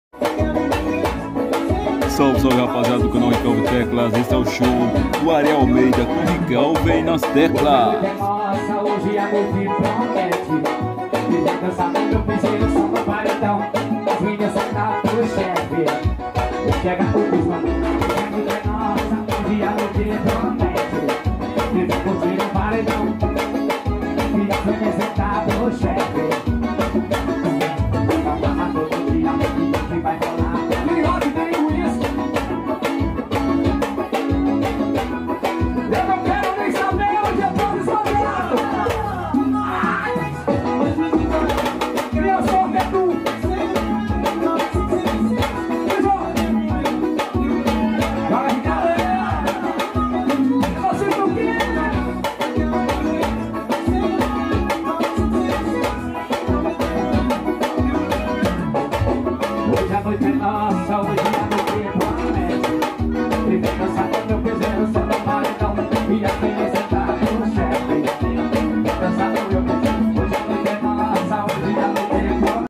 Ariel Almeida & Rikelvin Teclass em sítio do quinto Bahia 2023
Show em sítio do quinto Bahia Nordeste Brasil